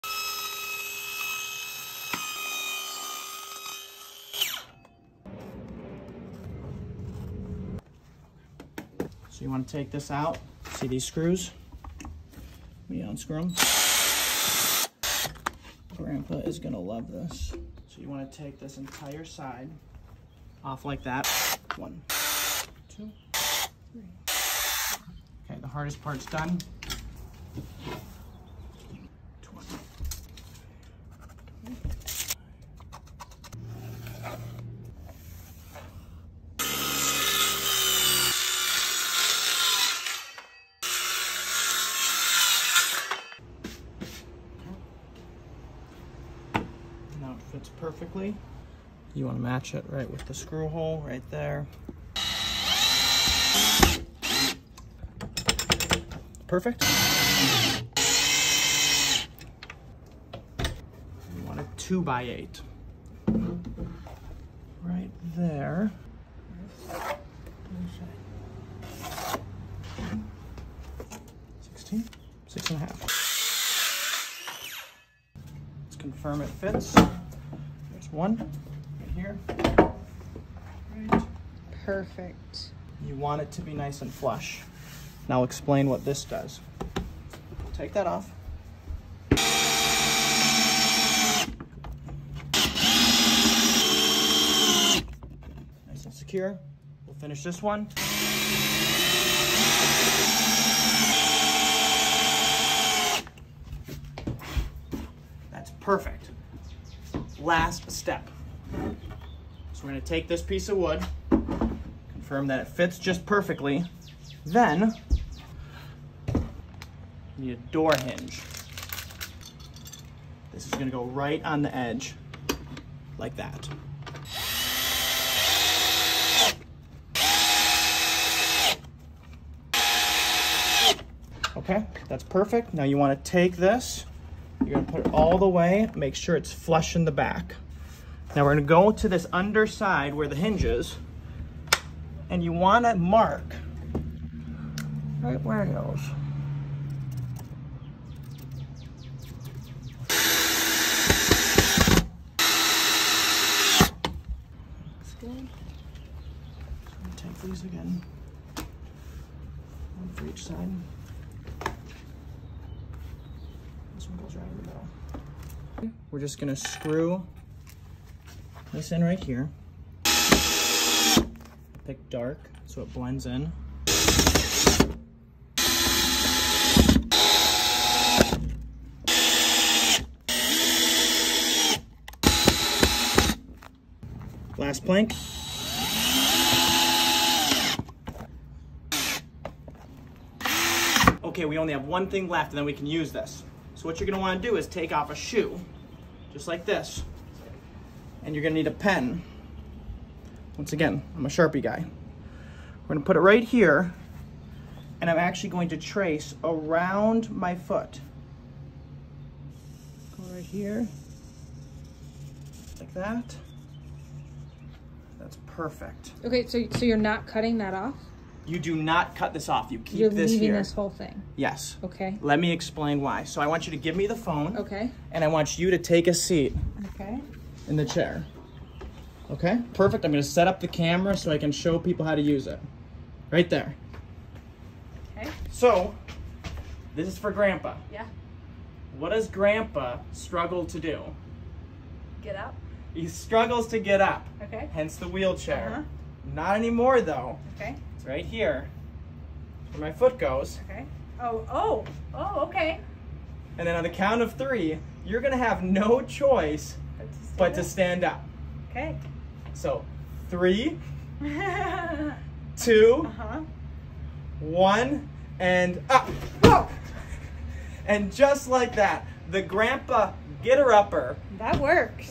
Man saves Grandpa with New Wheelchair invention!
This man invents a quick and easy solution to help your grandfather as he gets older.  Every kid and parent should be aware of this little secret!